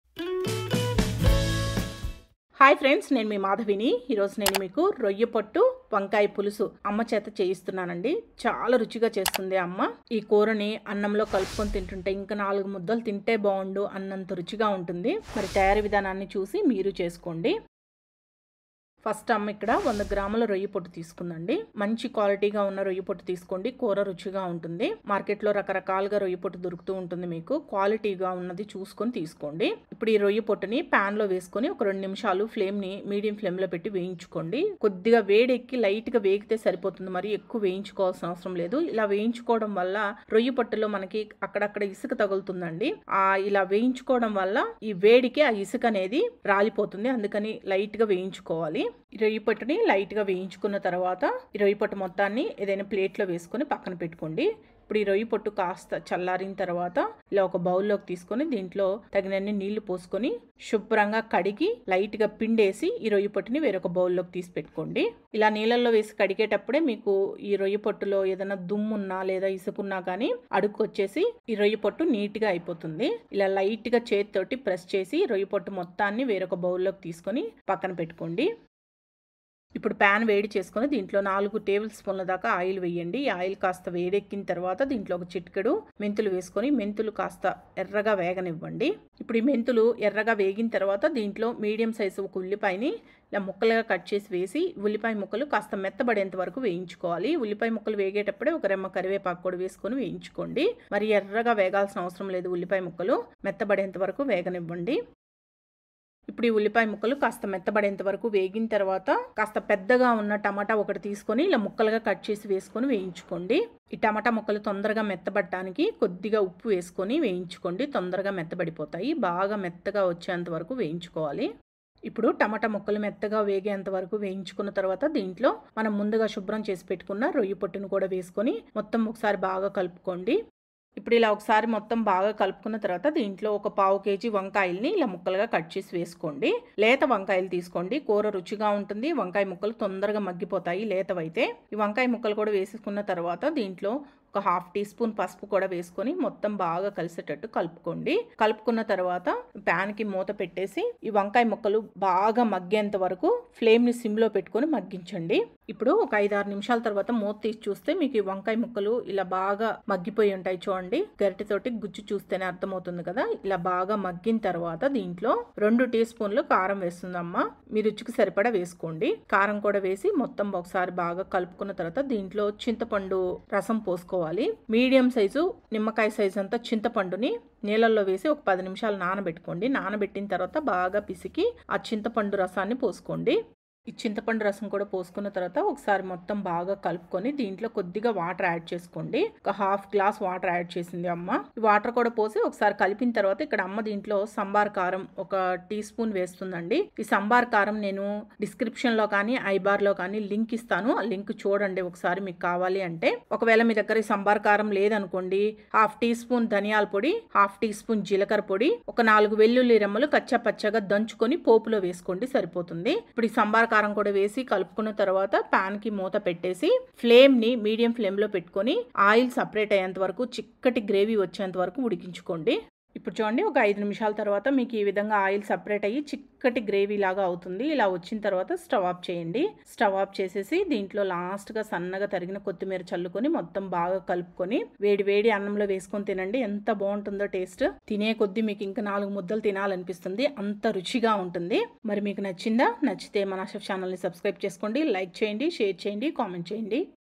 మా అమ్మ చేతి రొయ్య పొట్టు వంకాయ పులుసు నాలుగు ముద్దలు ఎక్కువే తింటారు Royya Pottu Vankaya Pulusu
Learn My mother Style Royya Pottu Vankaya Pulusu Recipe in Telugu.